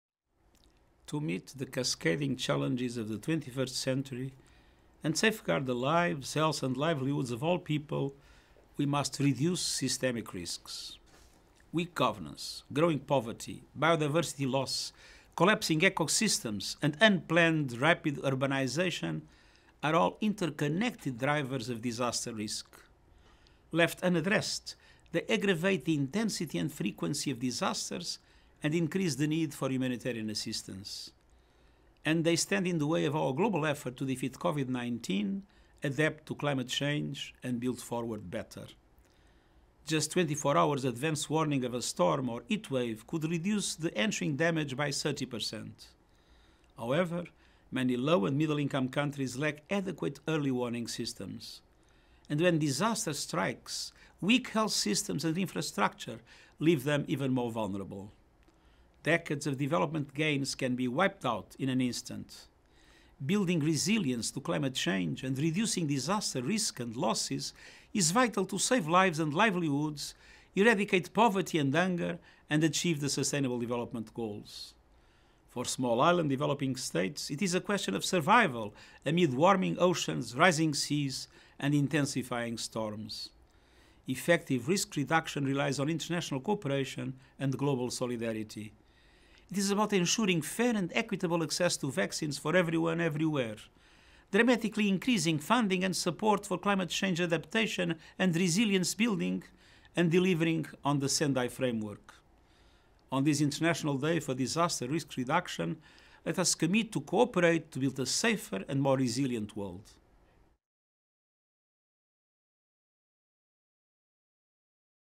国際防災の日（10月13日）に寄せるアントニオ・グテーレス国連事務総長ビデオ・メッセージ
次々と押し寄せる21世紀の課題に対応し、すべての人々の生命や健康、生活を守るために、私たちはシステミック・リスク（連鎖的に存在する多様なリスク）を軽減しなければなりません。

脆弱なガバナンス、貧困の拡大、生物多様性の喪失、生態系の崩壊、そして無計画で急速な都市化はいずれも、相互に関連している災害リスクの要因です。

これらを放置すると、災害は激甚化、頻発化し、人道支援のニーズが高まります。

そしてこれらの要因は、新型コロナウイルス感染症（COVID-19）に打ち勝ち、気候変動に適応し、より良い社会へ前進するためのグローバルな取り組みの妨げとなります。

暴風雨や熱波のわずか24時間前に事前警報を出すことができれば、その後の被害を30パーセント軽減することができます。

しかし、多くの低・中所得国には適切な早期警報システムがありません。

そして、災害が発生すると、保健医療システムやインフラが脆弱なこれらの国々は一層脆弱な立場に追いやられます。

数十年にわたる開発の成果が一瞬にして台無しになりかねません。

気候変動へのレジリエンス（強靱性）を構築し、災害リスクと被害を軽減することは、生命と生活を守り、貧困と飢餓を撲滅し、持続可能な開発目標（SDGs）を達成するために不可欠です。

海洋が温暖化し、海水面が上昇し、暴風雨が激甚化する中、それは小島嶼開発途上国にとって存続に関わる問題です。

効果的な災害リスク削減は、国際協力とグローバルな連帯にかかっています。

それは、あらゆる場所で、すべての人々がワクチンに公平・公正にアクセスできるようにすることです。

気候変動への適応とレジリエンス構築への資金拠出と支援を劇的に増やすことです。
そして、「仙台防災枠組」を実行することです。

「国際防災の日」にあたり、より安全でレジリエント（強靱）な世界を築くために協力することを決意しようではありませんか。